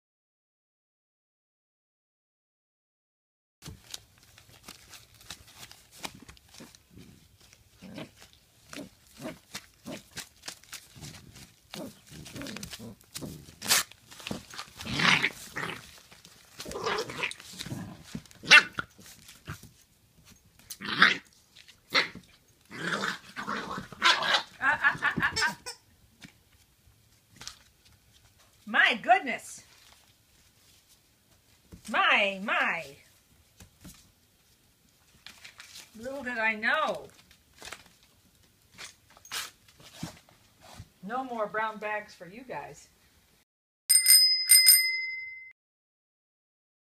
Brown Bag
Need a new puppy toy? Crinkle up a small brown bag and see what happens.